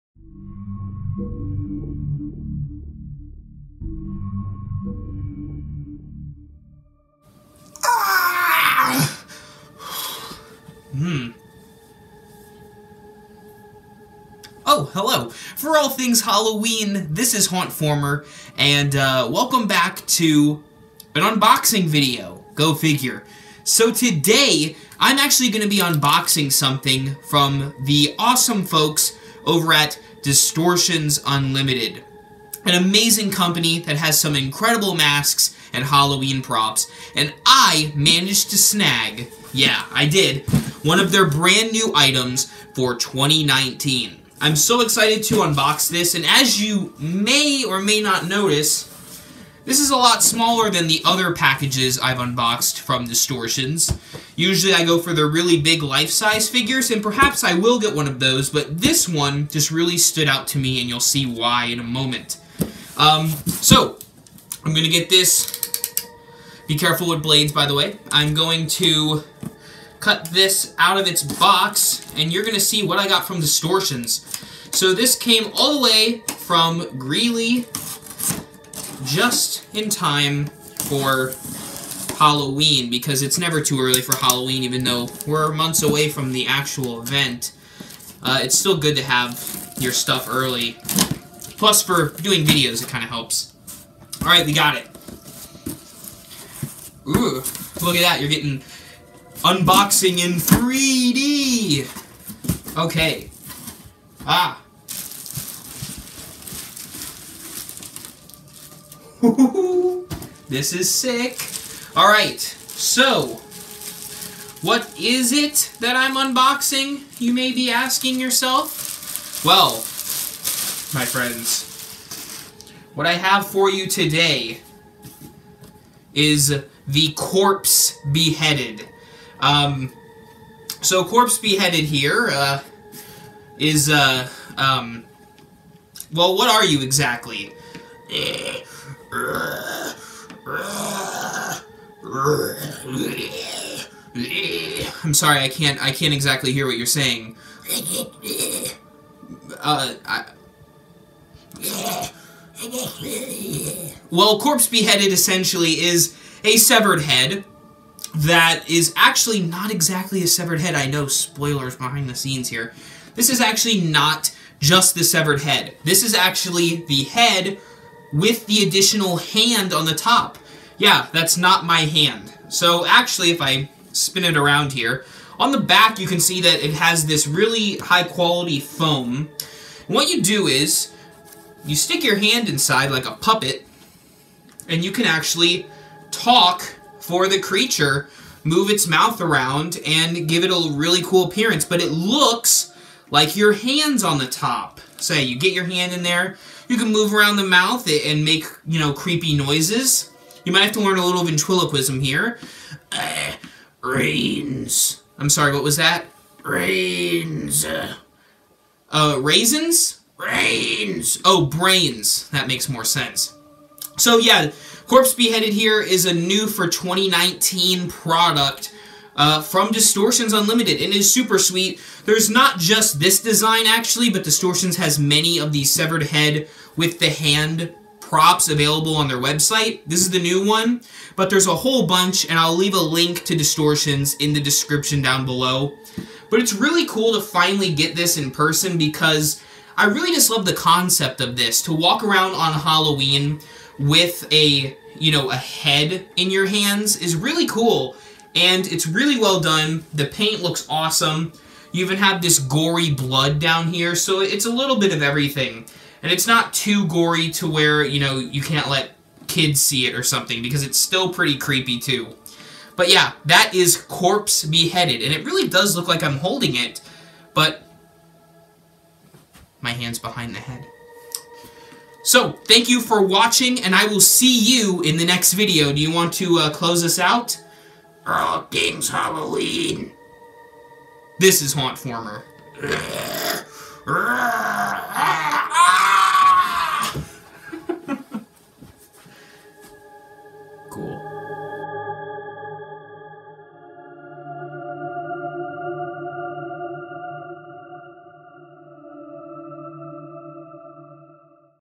Distortions Unlimited 2019 Prop UNBOXING
Distortions Unlimited is a leading innovator in the world of Halloween props, masks, and costumes. This year, I managed to snatch up an interesting little creep...